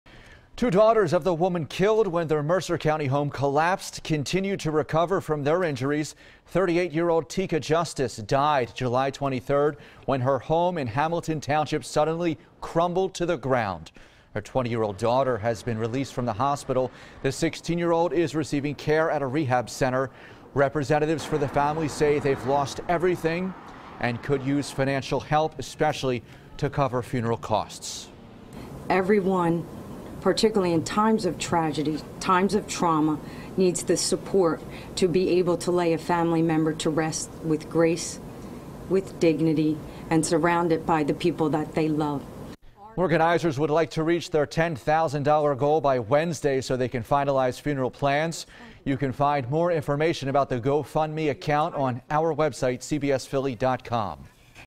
Family Involved In Deadly Hamilton Township House Collapse Asking For Financial Help
Two daughters of the woman killed when their Mercer County home collapsed continue to recover from their injuries.